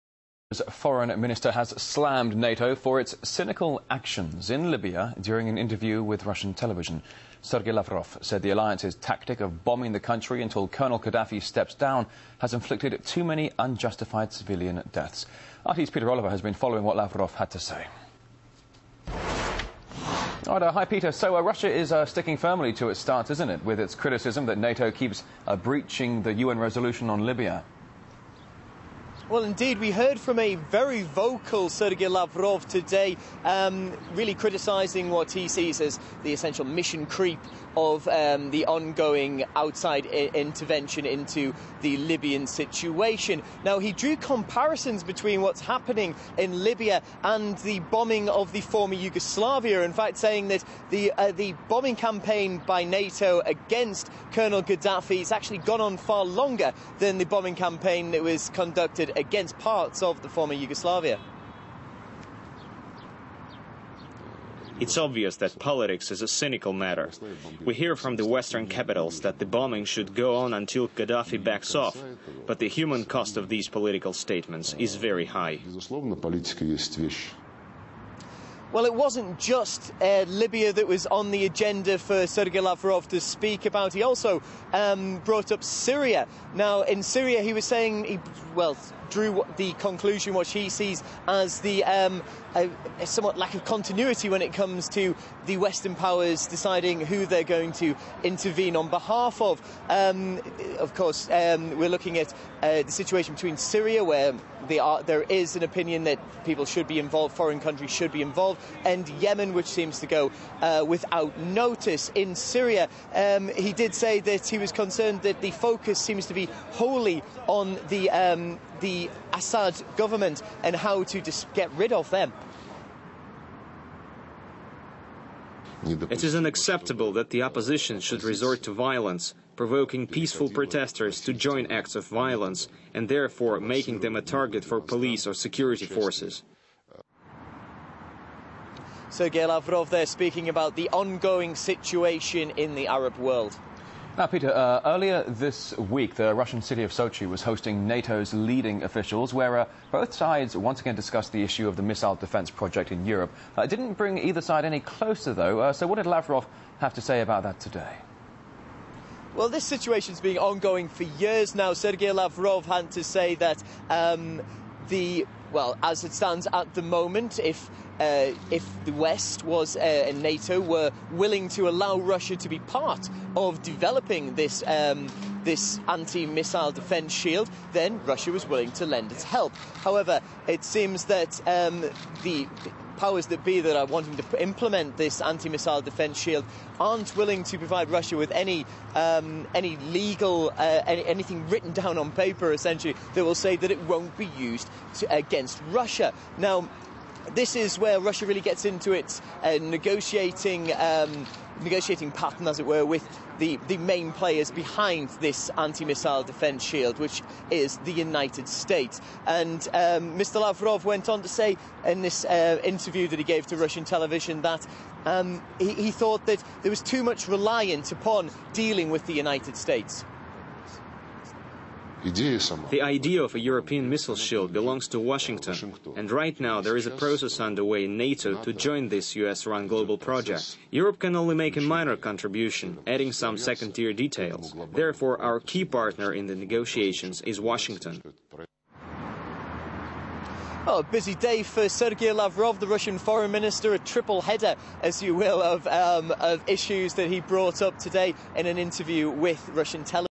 No quick solution to Libyan conflict -- Lavrov.
credit to rt